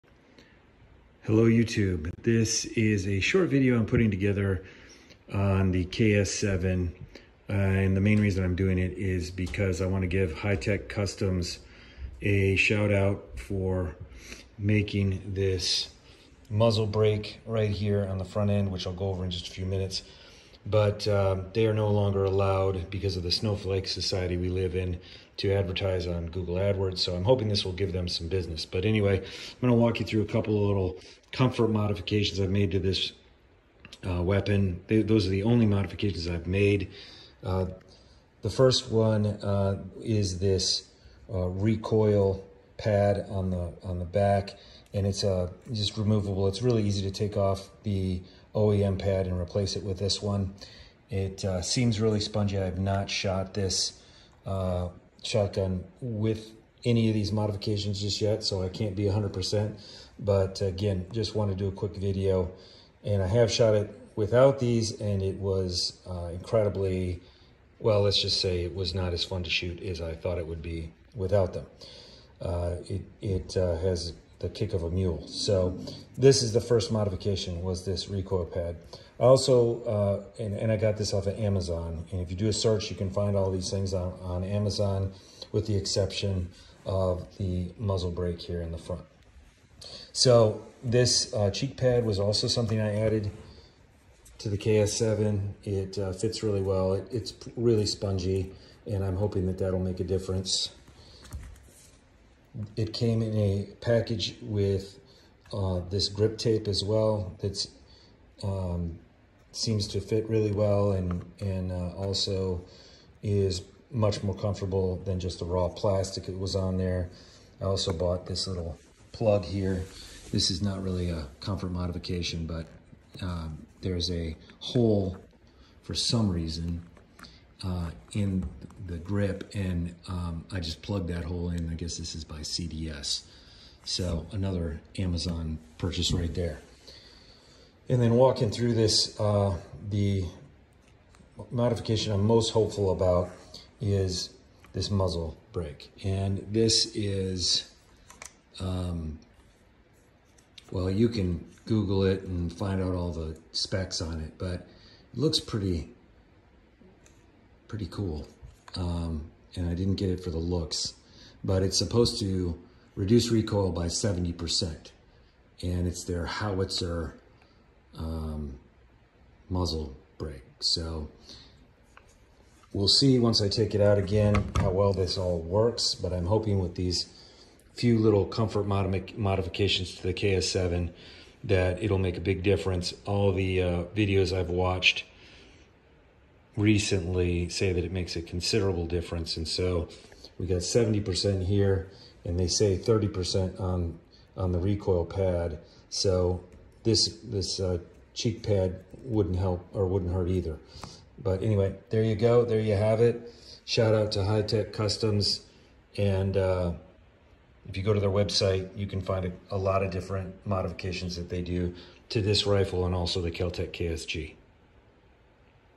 KelTec KS7 bullpup comfort modifications
FYI, I did not show a safety check on this weapon in my video. I do not think I am putting you in danger by not showing you that it is unloaded and safe. So for all you who like to play youtube-gun-safety Sally and throw your two cents in , don’t.

Enjoy this short video on some comfort modifications I just made to a KS7 by KelTec. Shout out to @Karl Hi-Tech. He was very helpful during my search to make this shotgun more enjoyable.

High Tech Custom Concepts makes the muzzle brake in this video and they promise a 70% reduction in recoil with this. The KS7 has quite a kick so I am hopeful.